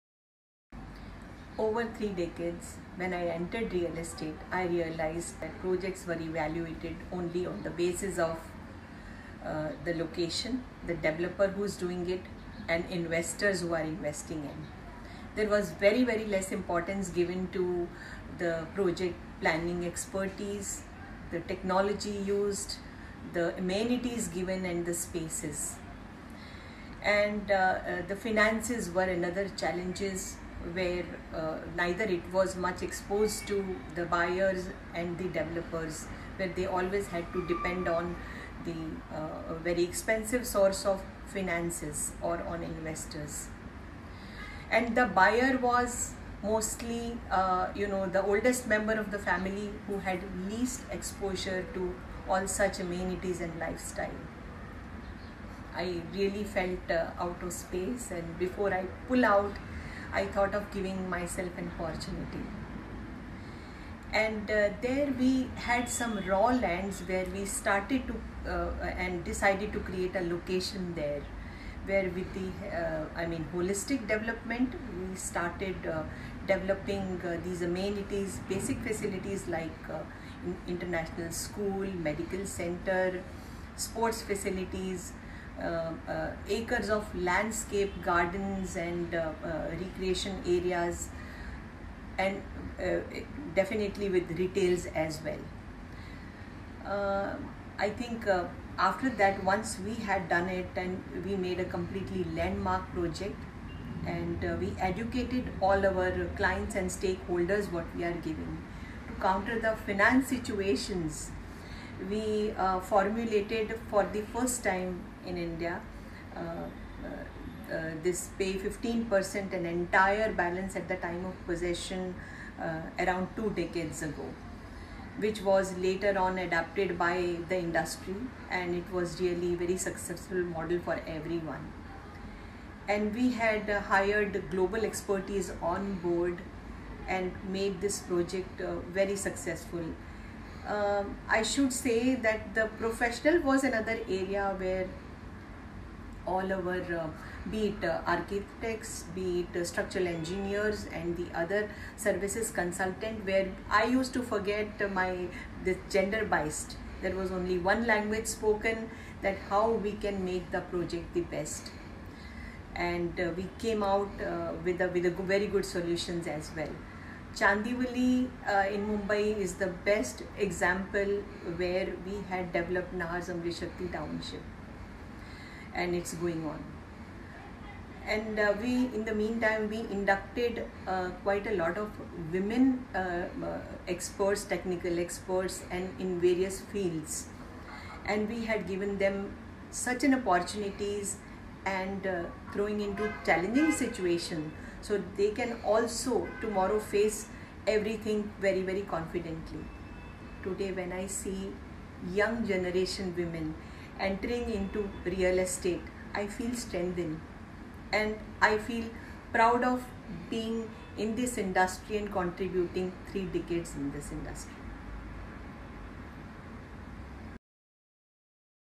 Happy International Women's Day- Ms. Manju Yagnik
On the occasion of International Women's Day, Ms. Manju Yagnik, VCP , Nahar Group and Senior Vice President, NAREDCO Maharashtra talks about her journey and contribution in the Indian Real Estate Sector.